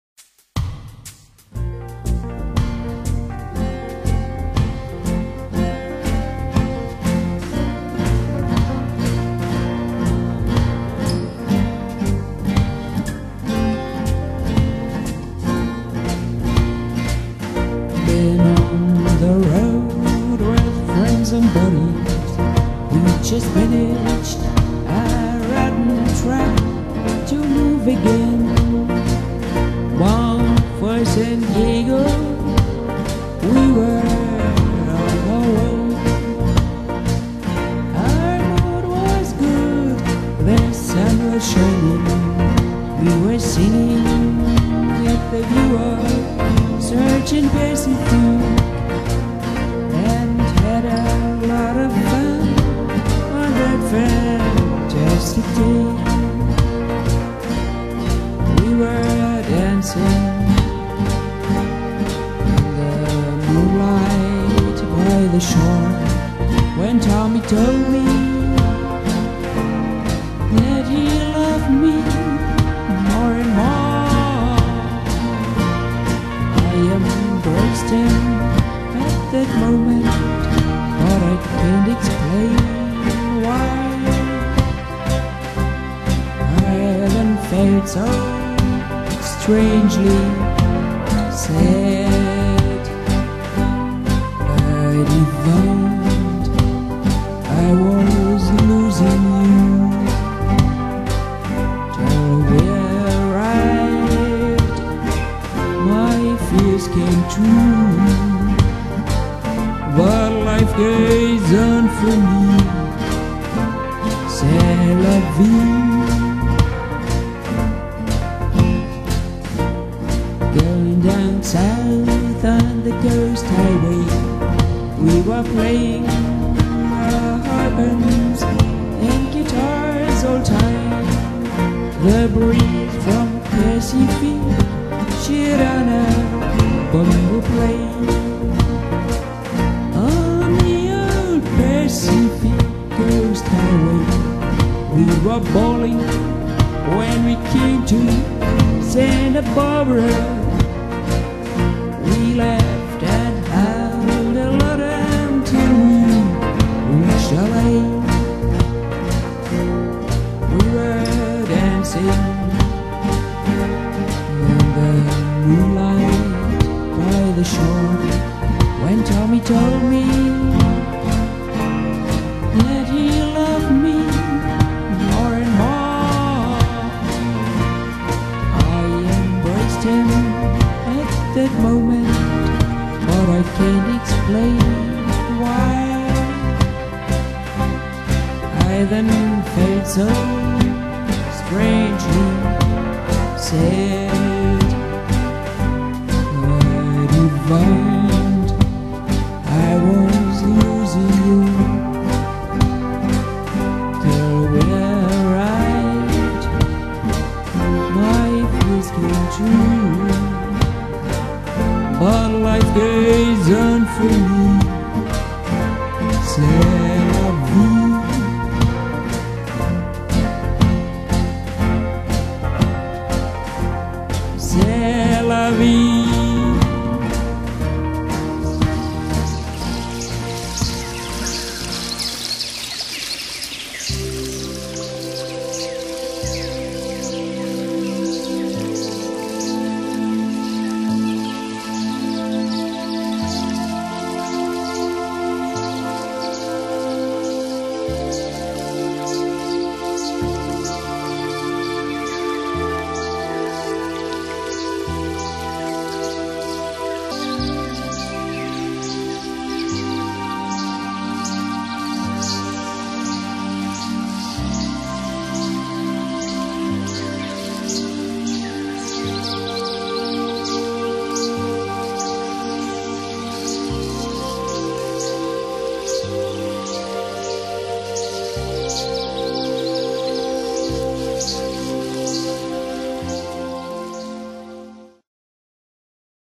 Horse Photography - Original Song by Carola Rost
Beautiful pictures of wild and domestic horses.